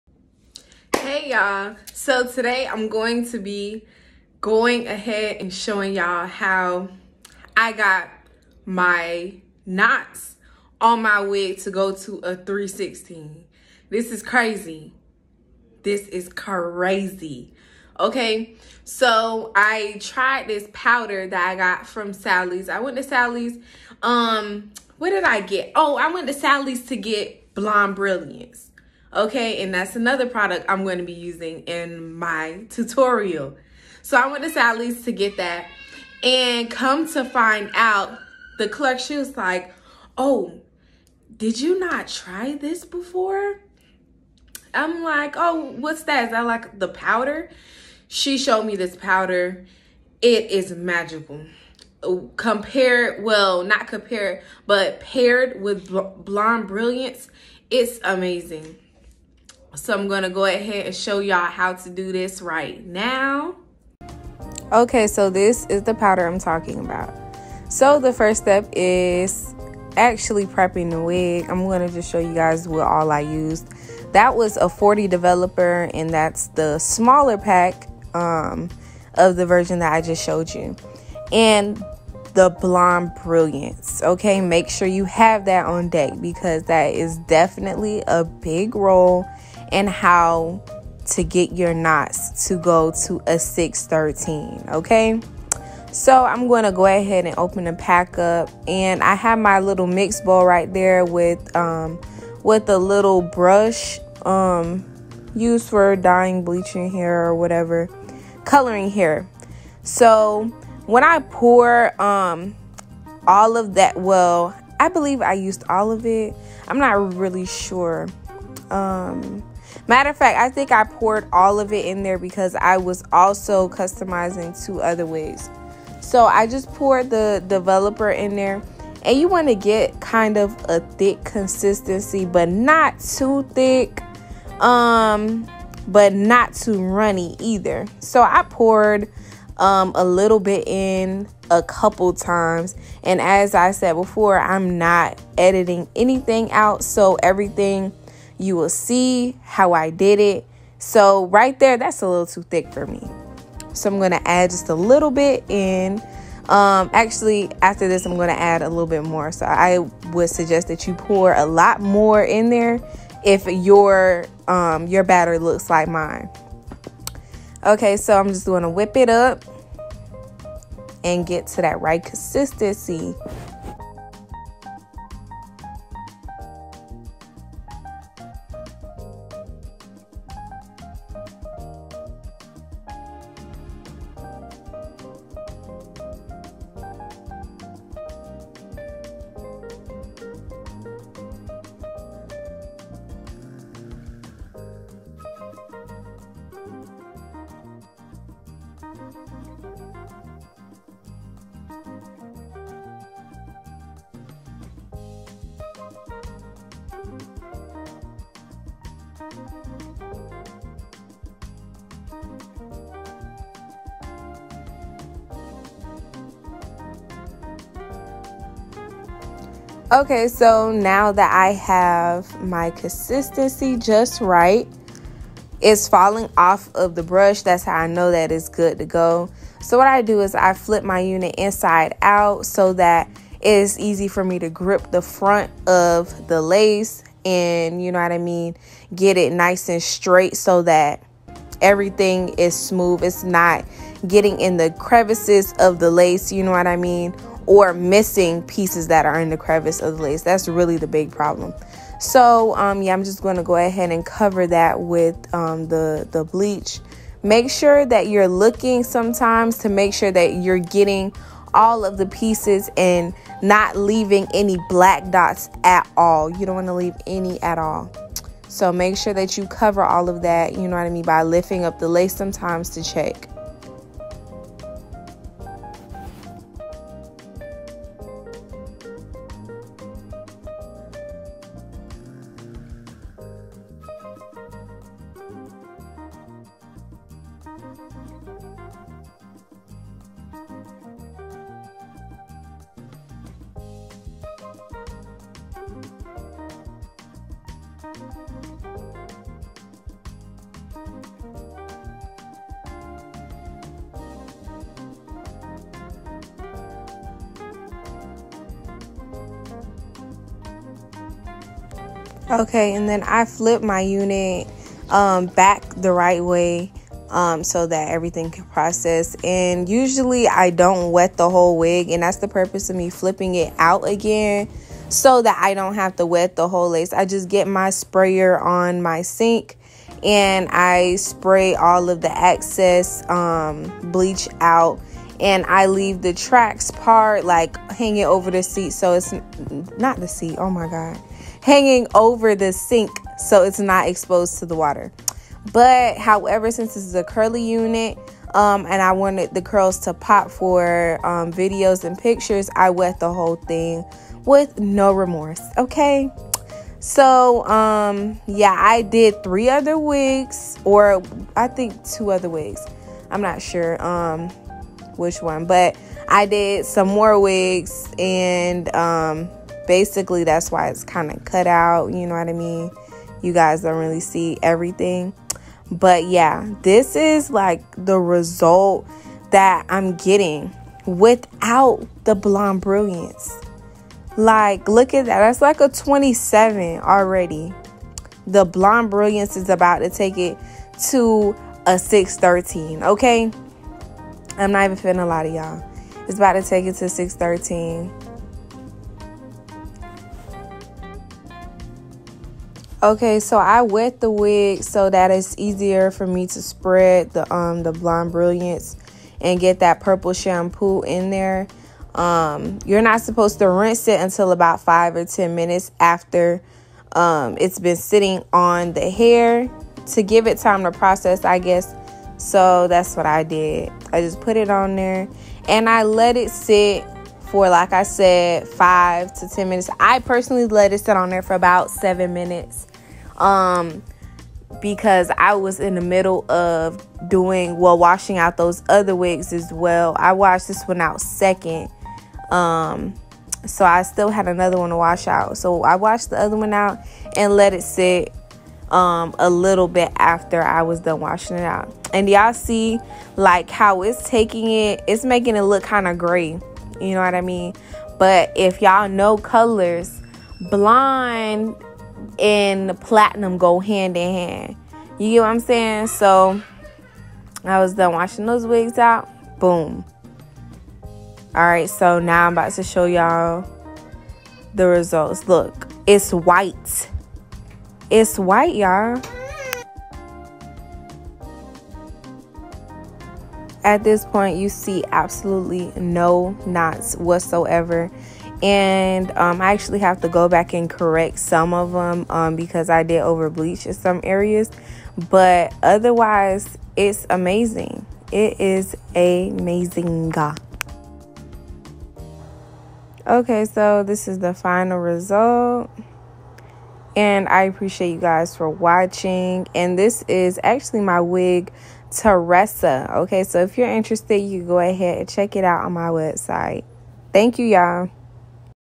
How bleached my knots to a 613 ! 😱
Bleaching my knots on my wig “teressa” to a 613! Something I didn’t think was possible proved to be possible !!

Products:
Zotos age beautiful ultra bond
40 developer
Blonde brilliance copper toner